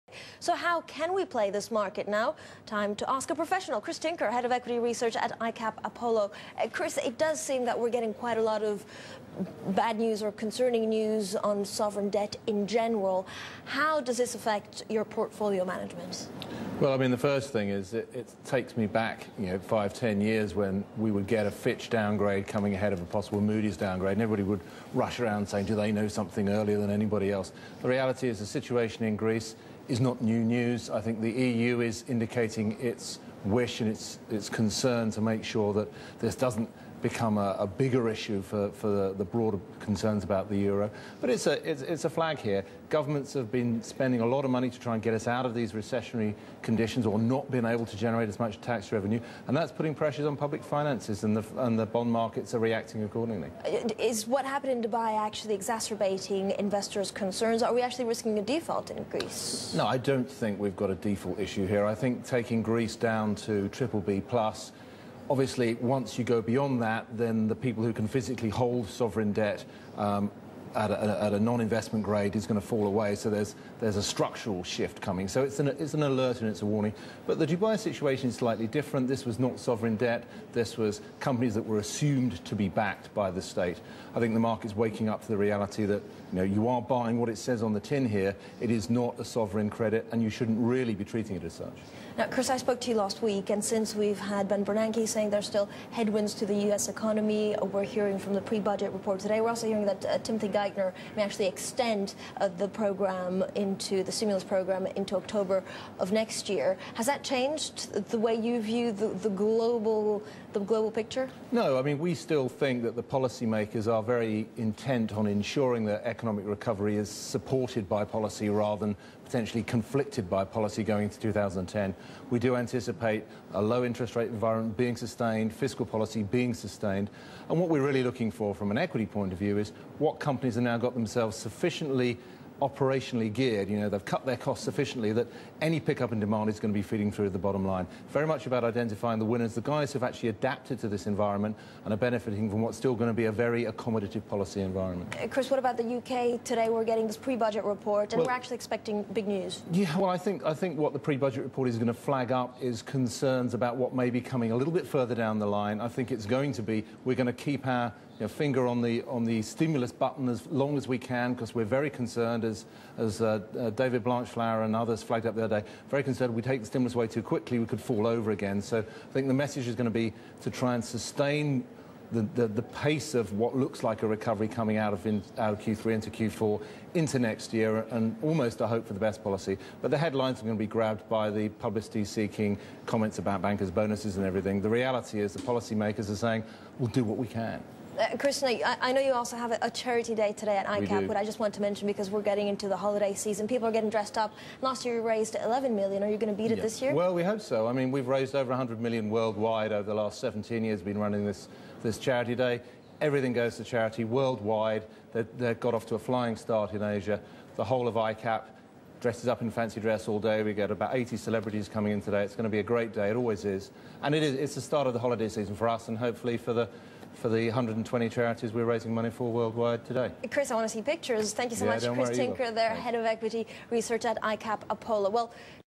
ICAP Apollo's Tinker Doubts Greece Facing Debt Default
Dec. 9 (Bloomberg) -- Chris Tinker, head of equity research at ICAP Apollo Plc, discusses concerns about the fiscal situation in Greece.
     Speaking with Bloomberg's Francine Lacqua in London, Tinker also discusses the U.K. pre-budget report. (Source: Bloomberg)